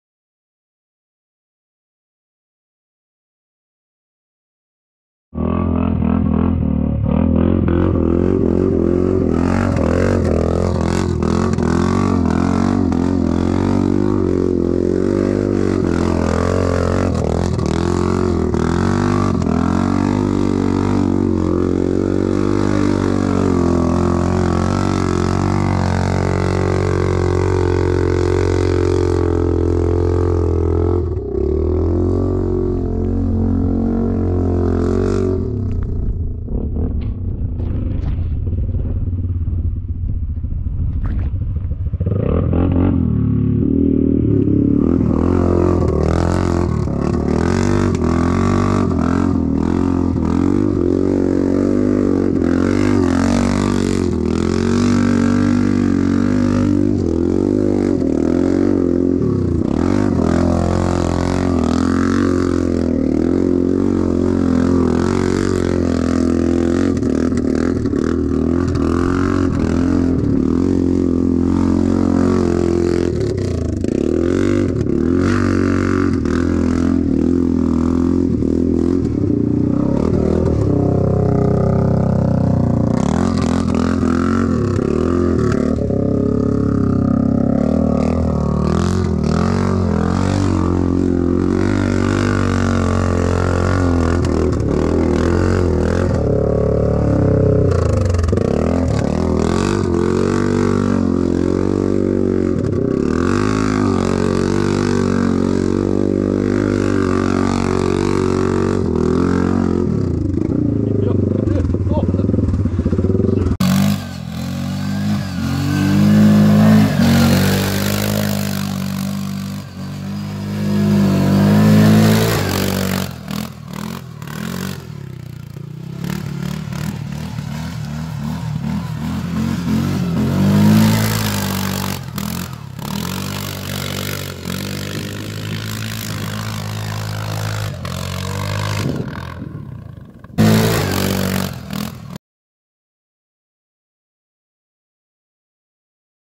Comparison of the CF MOTO 520L vs 520s quad. Fun in the sand mine
Comparison of the CF MOTO 520s vs 520L quad. Fun in the sand mine